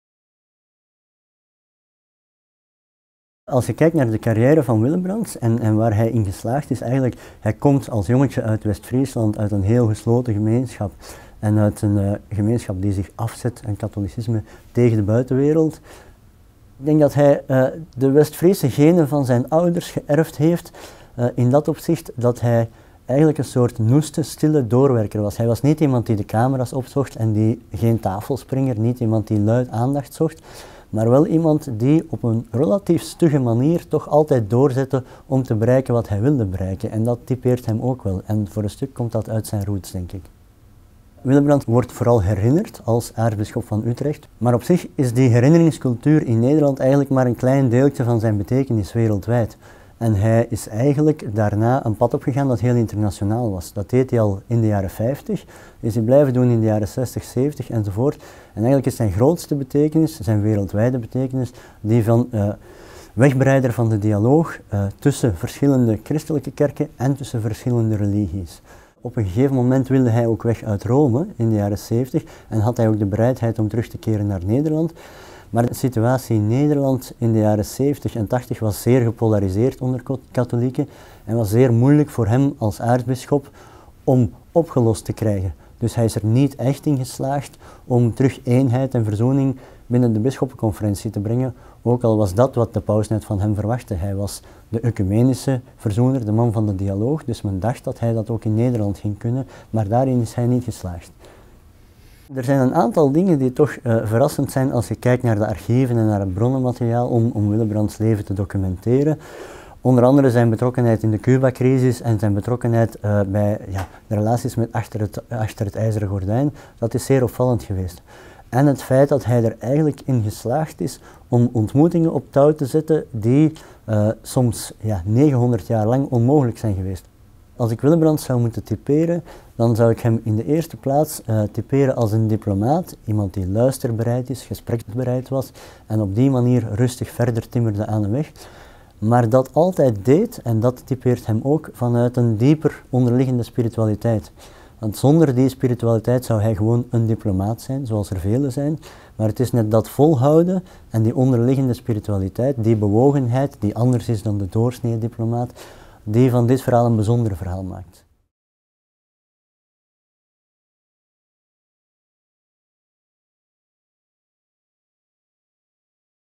Boektrailer: 'Johannes Willebrands - Een leven in gesprek', geschreven door Karim Schelkens
De biografie 'Johannes Willebrands - Een leven in gesprek' is verkrijgbaar via de volgende

Het verhaal van Jo Willebrands is dat van een Nederlander die tijdens de Koude Oorlog doordrong tot ver achter het IJzeren Gordijn, vertegenwoordigers van Moskou ontmoette en in de achterkamers van de Cubaanse rakettencrisis verzeild geraakte. Van iemand die Oekraïense leiders uit de Goelag wist te krijgen en mede de hand had in de Vaticaanse diplomatie die in 1989 de Berlijnse Muur deed vallen.